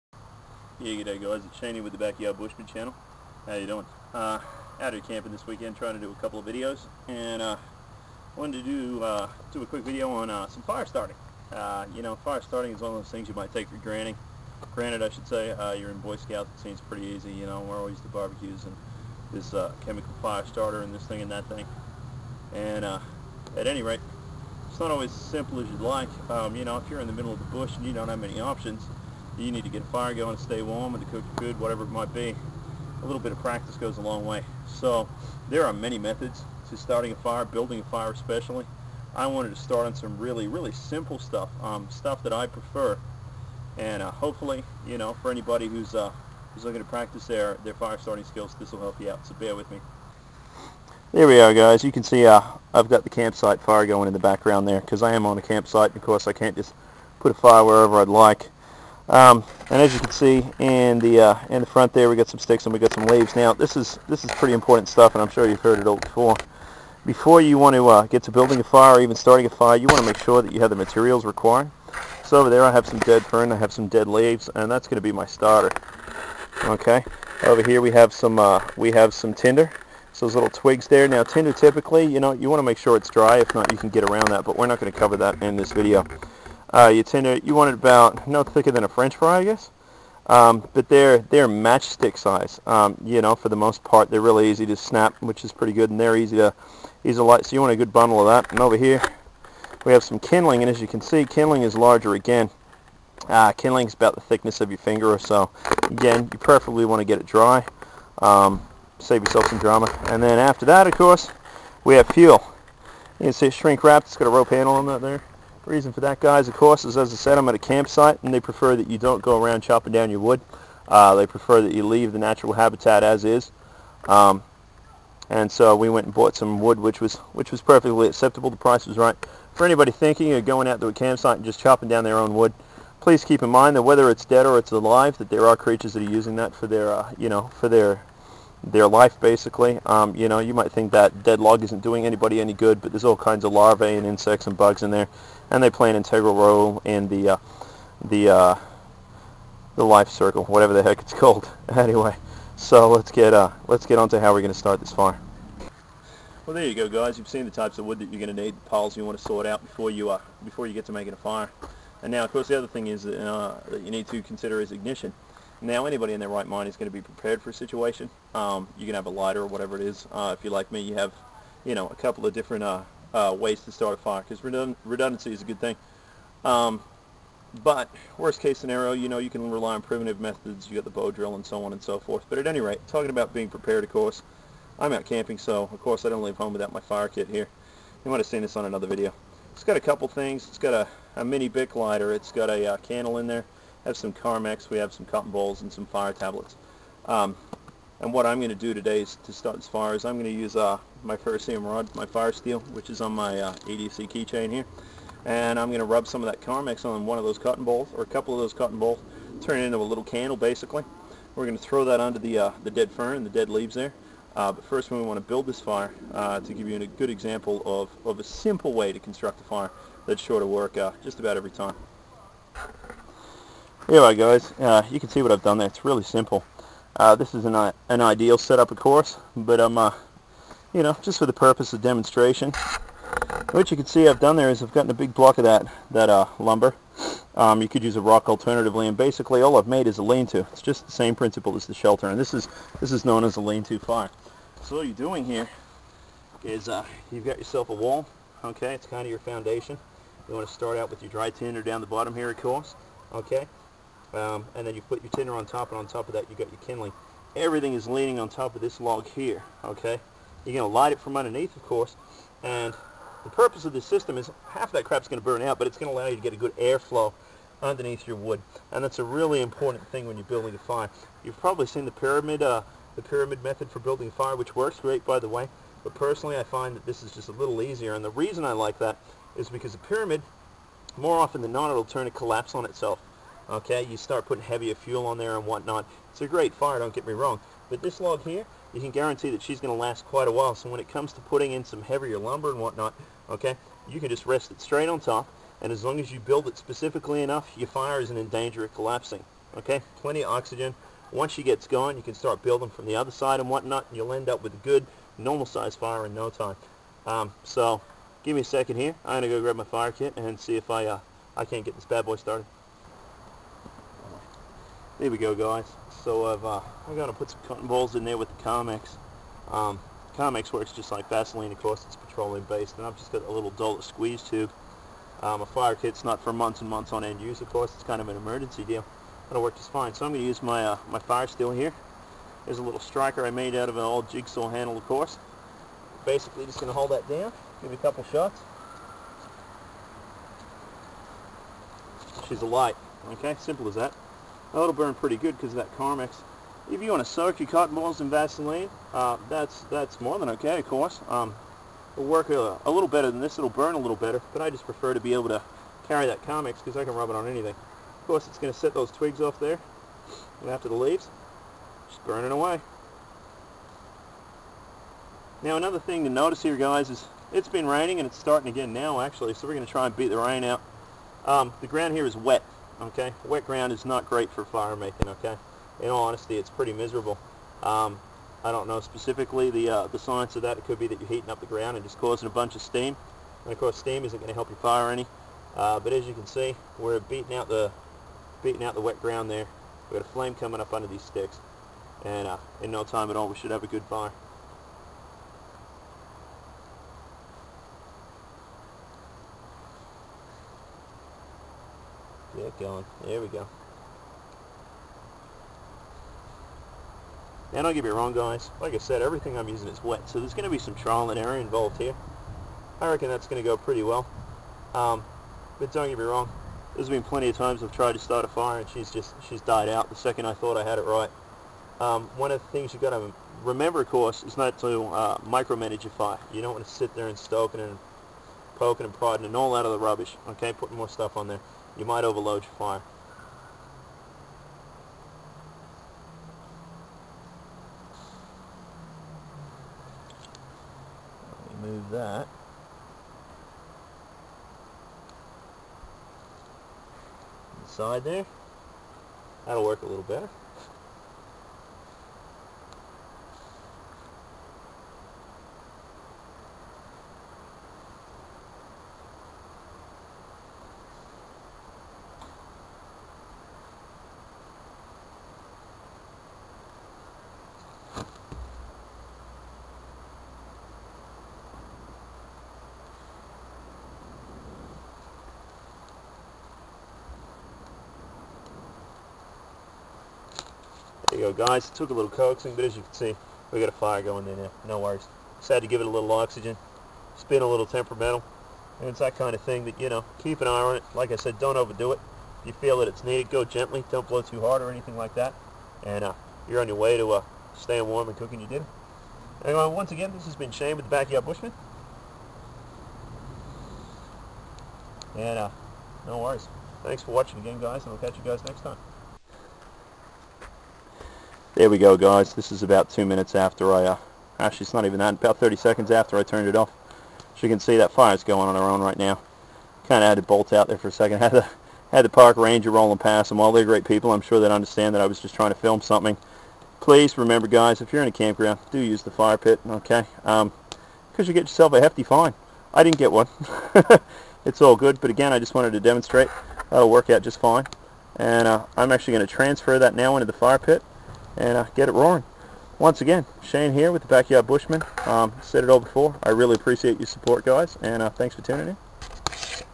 Fire Tips 1 & the lean-to fire
Some basic tips for firestarting and one method of building your fire.